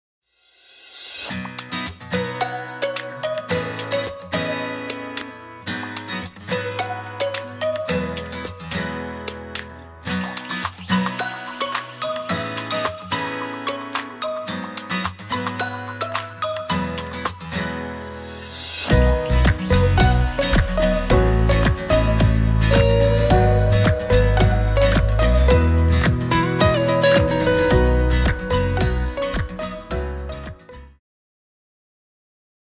Decantación de Vinos - Técnicas Servicio
Como decantar un vino para eliminar posos e impurezas